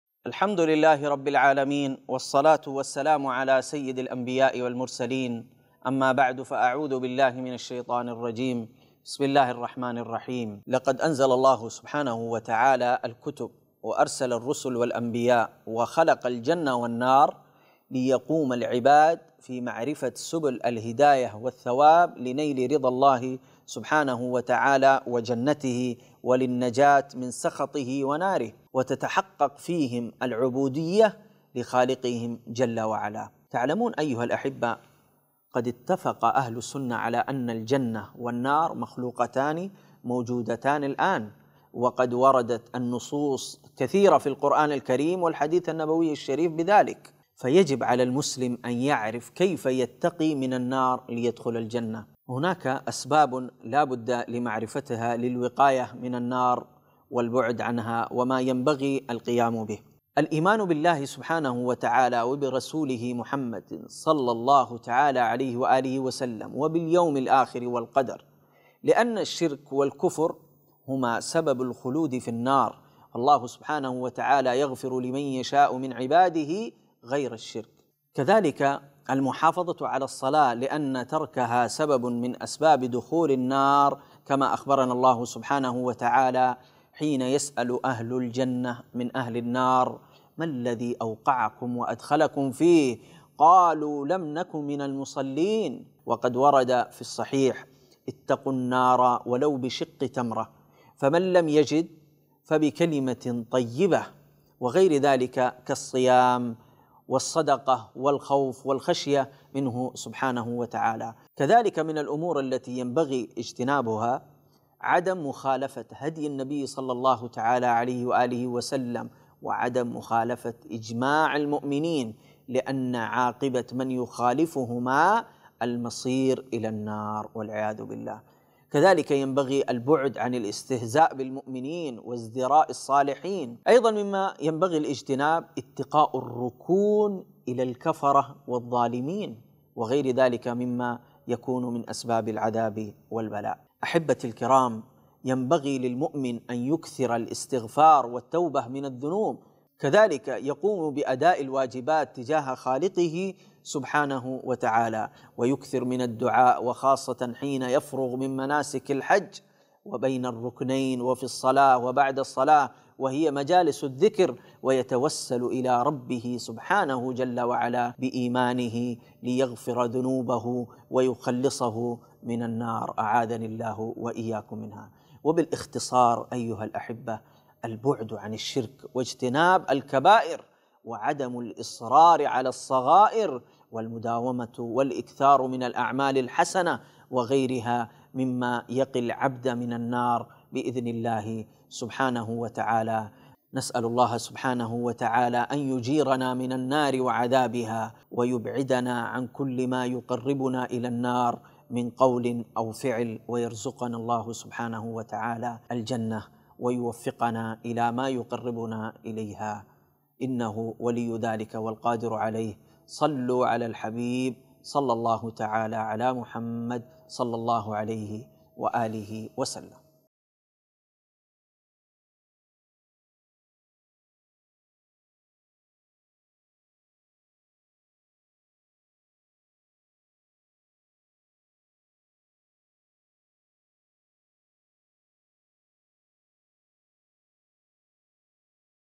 اتقوا النار || الشيخ عبد الله المدني
كيف يمكننا أن تحفظ نفسك من النار التي وقودها الناس والحجارة يوم القيامة ويجب علينا أن نخافها ونحذرها،كيف ذلك؟ شاهد الفيديو التالي

●▬▬▬▬▬▬▬▬▬▬▬ஜ۩۞۩ஜ▬▬▬▬▬▬▬▬▬▬▬●